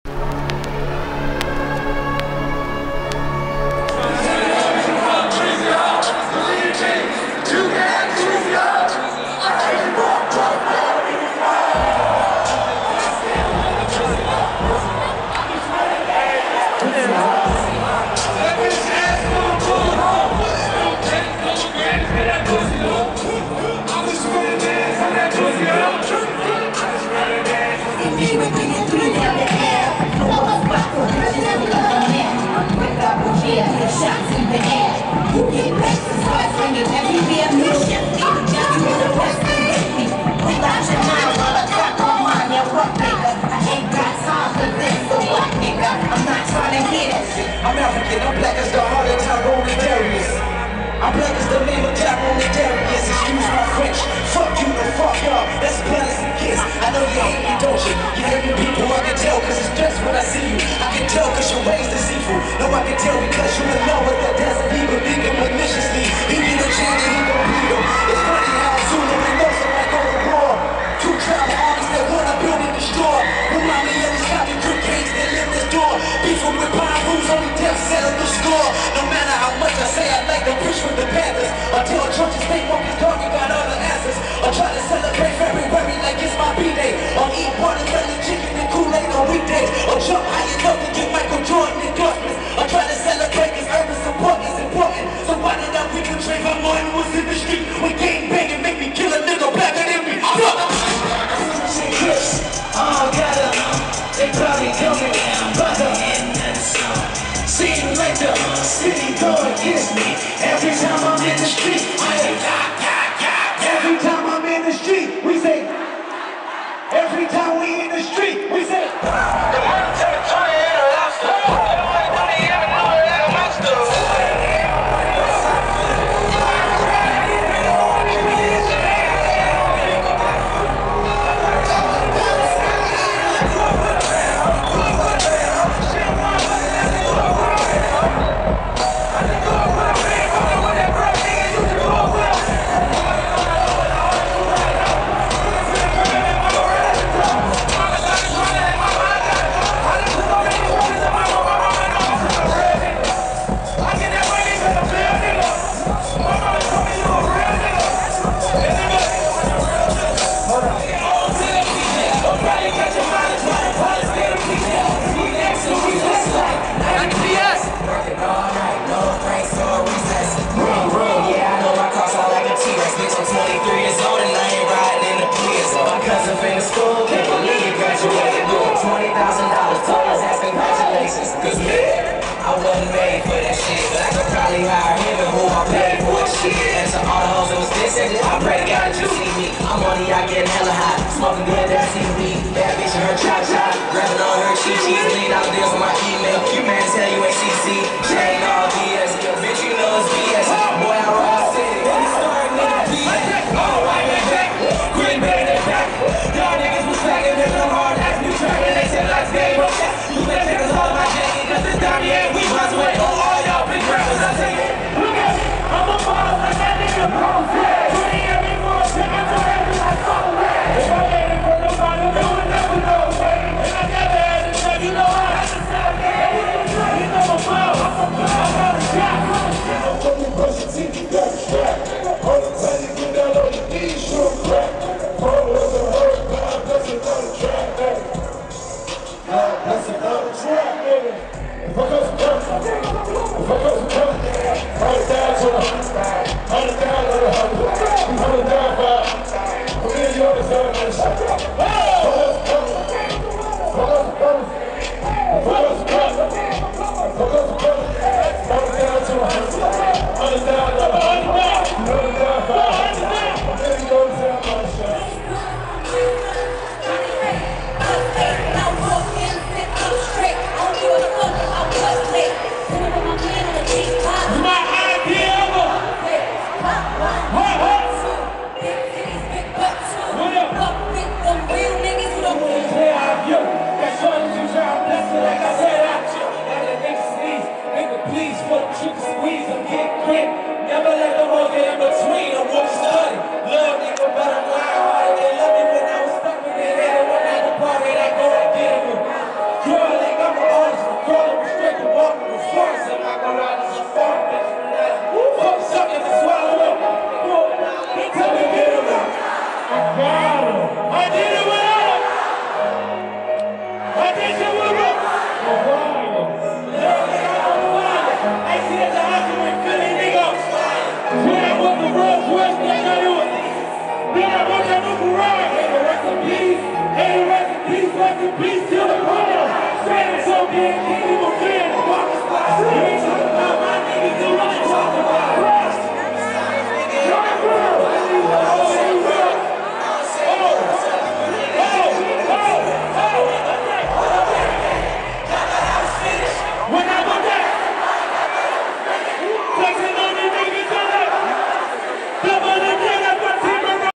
French Montana, Chris Brown, Meek Mill, Nicki Minaj & more @ Summer Jam 2015
On behalf of KYITL I want to thank HOT 97 for bringing us out to the best hip hop event in the world thank you!!